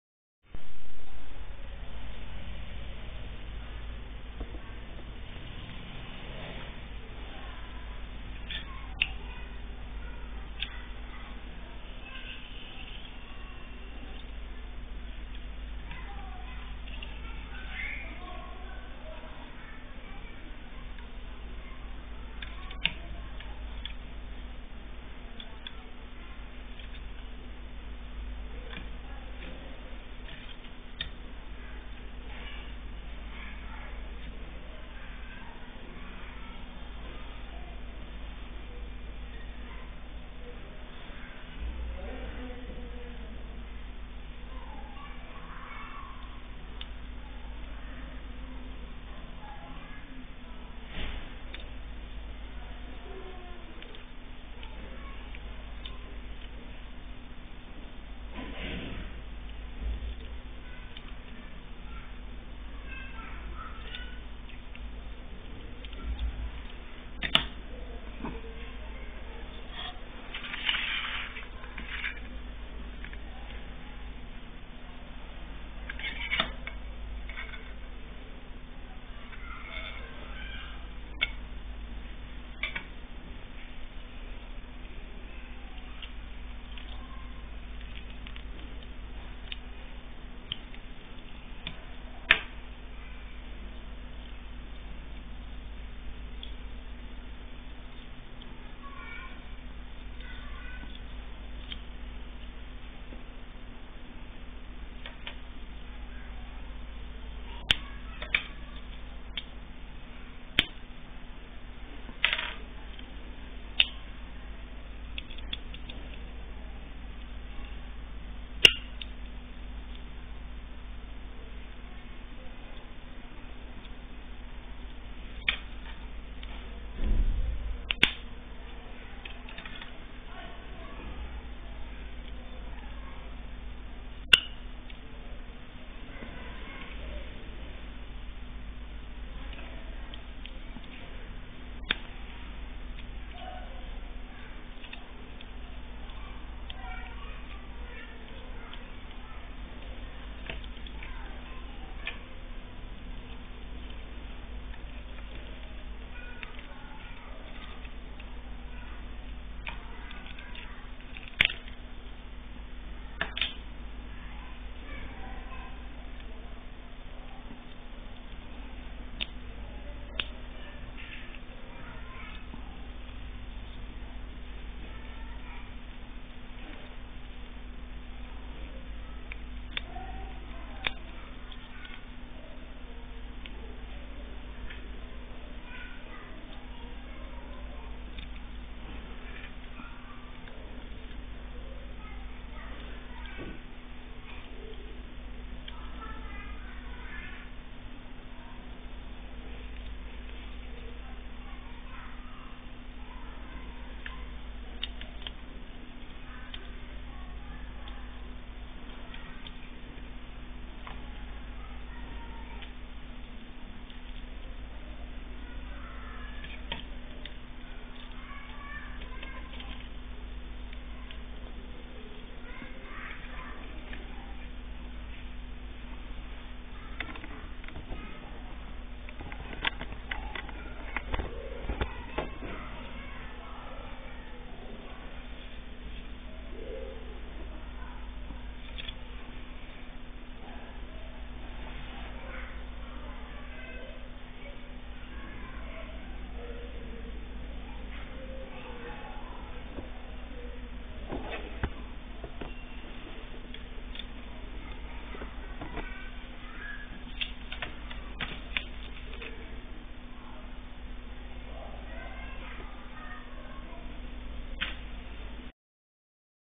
壯烈牺牲 2
去年搬家，其中沒能帶走的一部份高逹摸型(T ^ T)